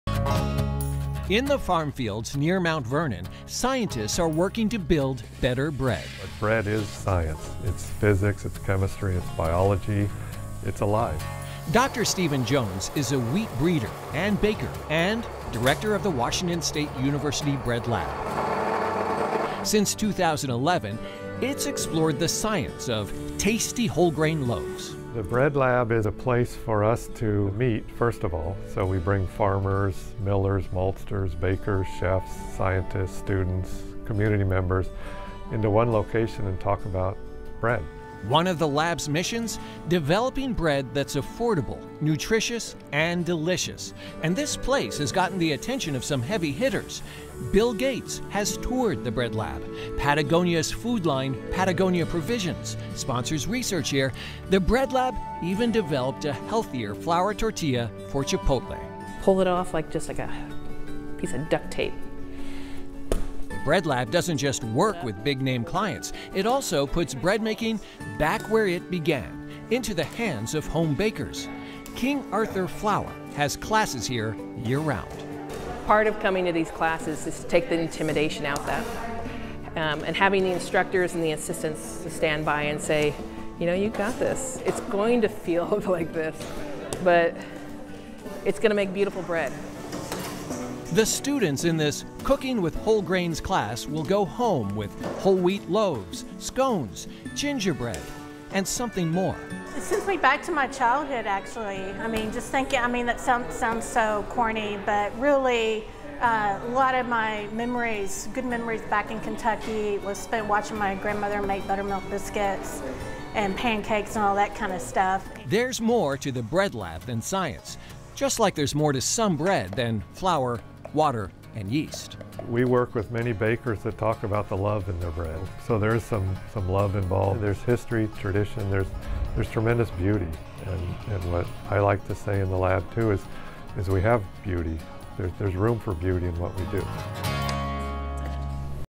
Skagit Valley Bread Lab devoted to baking better bread
'Bread is science. It's physics, it's chemistry, it's biology - it's alive,' said Dr. Stephen Jones, wheat breeder, home baker, and director of The Washington State University Bread Lab.  Since 2011, this place has explored the science of tasty whole grai